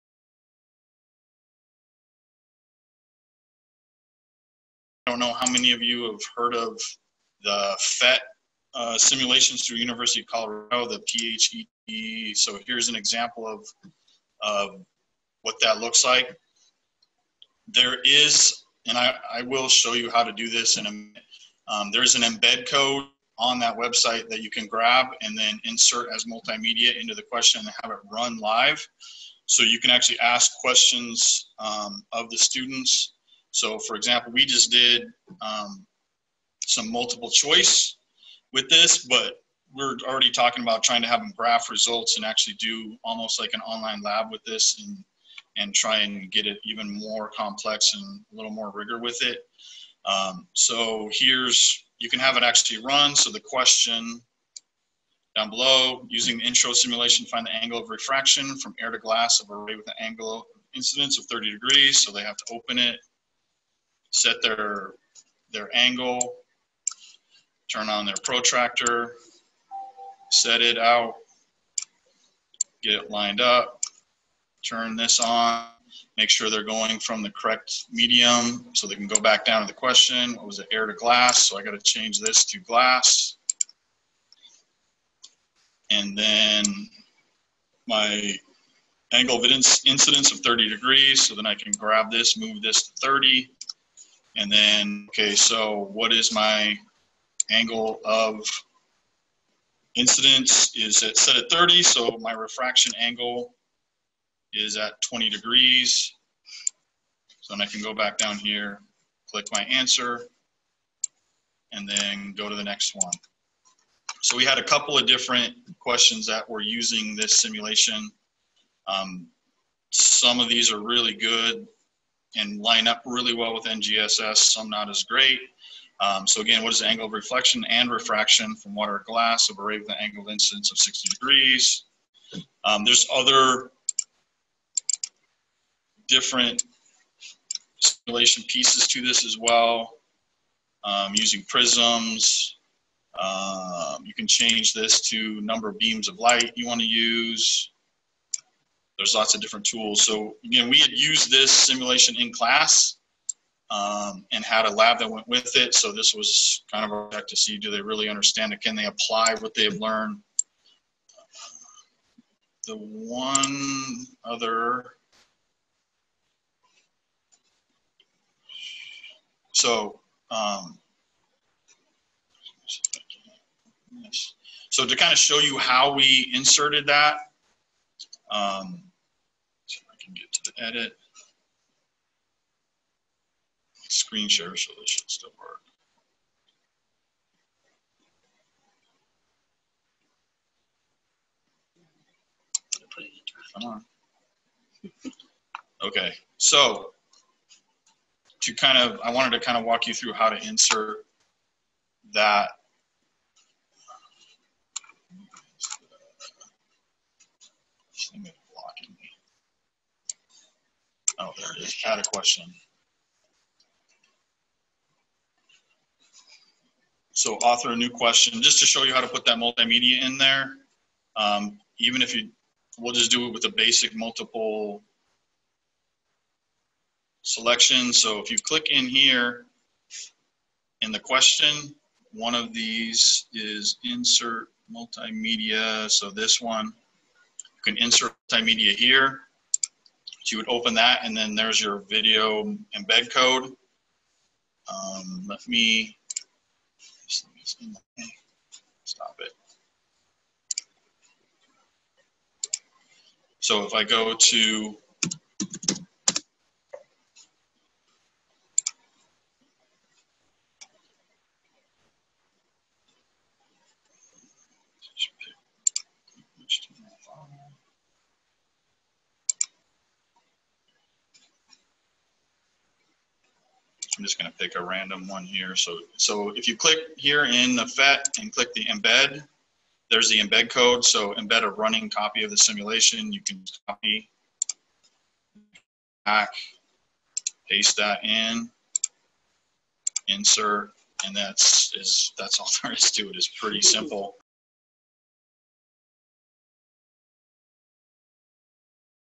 Add PhET Simulations to Your Edulastic Formative Assessments
Science teachers can add PhET math and science simulations to Edulastic assessments. Jason Anderson, 8th Grade Science Teacher in Clovis, CA walks through a sample simulation and how to add the simulation to your digital assessment.

to try it for yourself!